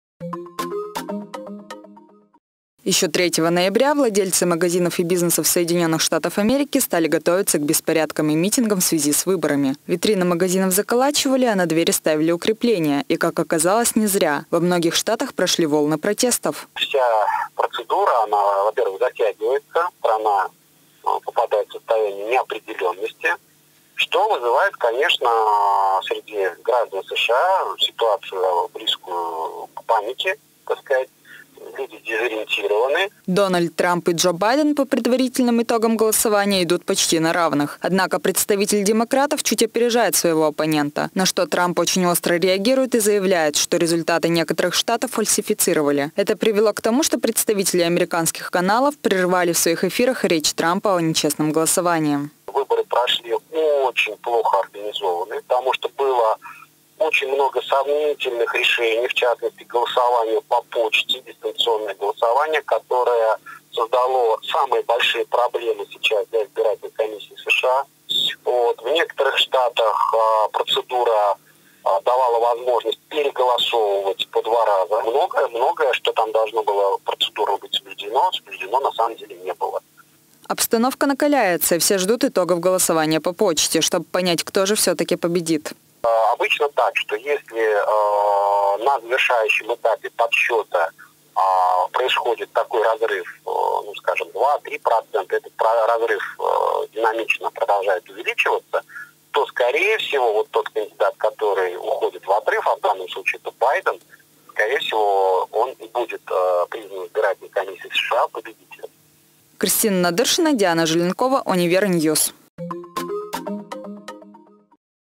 Чем закончатся выборы в США?
Митинги, погромы, выключение телеканалами речи Дональда Трампа и многое другое сопровождает президентские выборы в США. Подробнее об этом расскажем в нашем следующем сюжете.